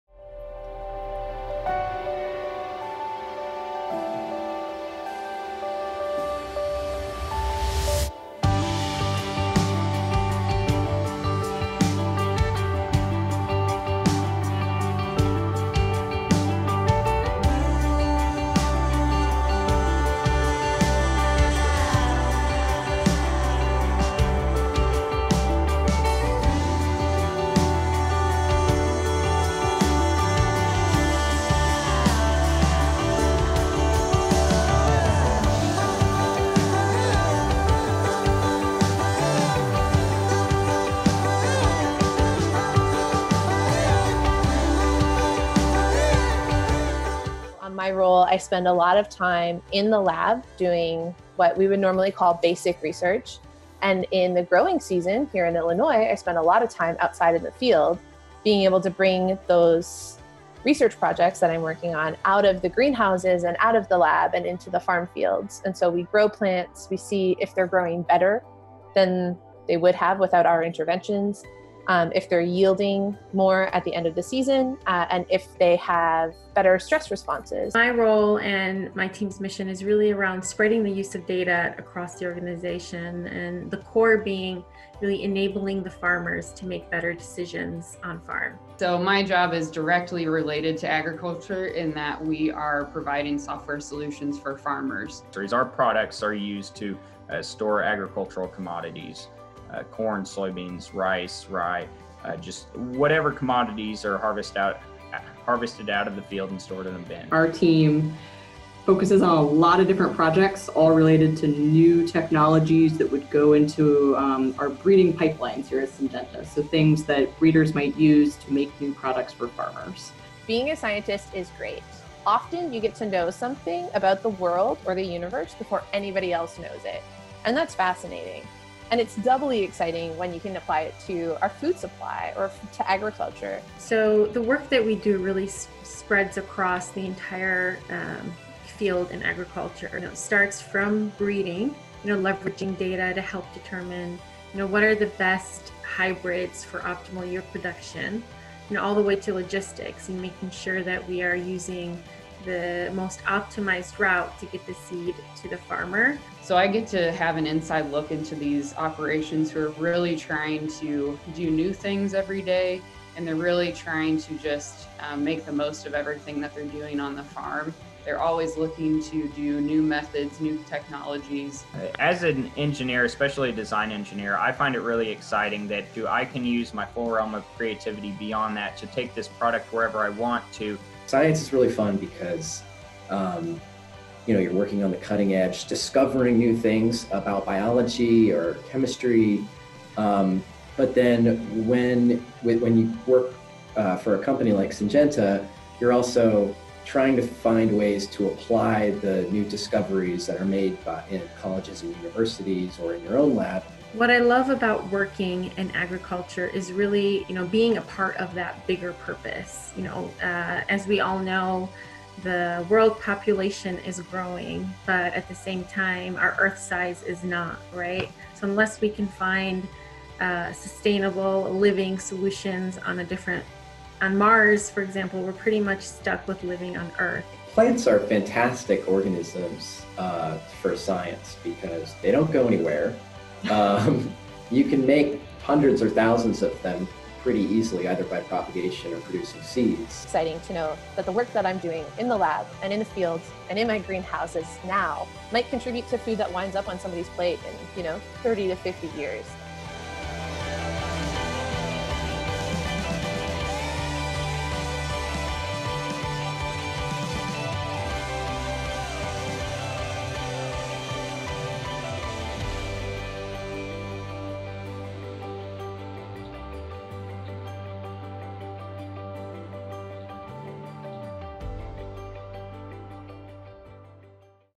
Exciting Careers in Agriculture That May Inspire You
The Illinois Soybean Association spoke with a few professionals working in the agriculture industry to learn more about the diverse array of careers available. From scientists to data analysts and engineers, there are numerous exciting possibilities to consider.